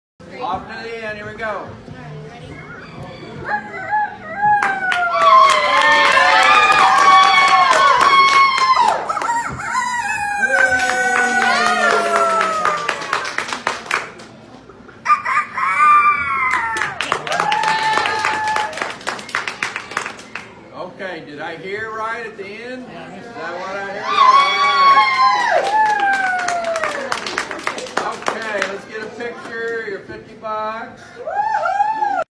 First winners for the Costume and Crowing contest 2019 Cackle Hatchery Chicken Festival
Here's a little snippet with the final three crowers